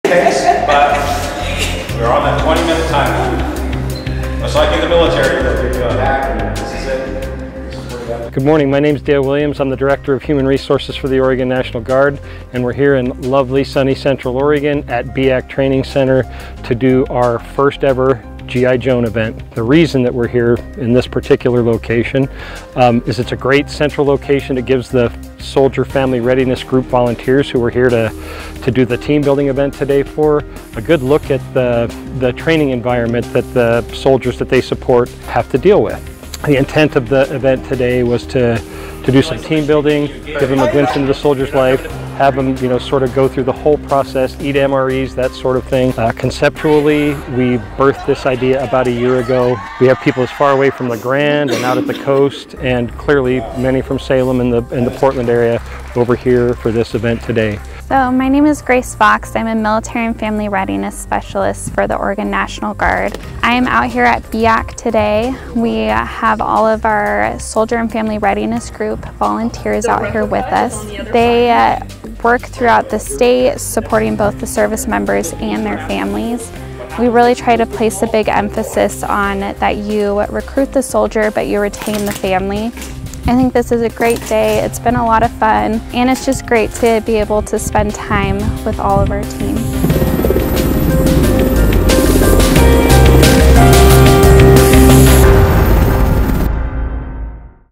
G. I. Joan Day
G.I. Joan Day was a Soldier and Family Readiness Group  (SFRG) Volunteer team-building event at Biak Training Center in Redmond on Feb 25, 2023. The training's purpose was to give SFRG volunteers an understanding of Soldiers' experience in the Oregon National Guard.

The event mimicked Army training with several Situational Training Exercises (STX) that required the volunteers to take turns being the leader and implementing troop-leading procedures (TLP). Also, The day exposed volunteers to Meals Ready to Eat (MRE) for lunch, a Blackhawk helicopter orientation, simulated firing M4s, the wear of combat equipment, and many other military activities.

The Soldier and Family Readiness Group is a critical component of mission readiness. As a command-sponsored organization, the SFRG is essential to network, communicate, support, and prepare our Soldiers and Family members for military life.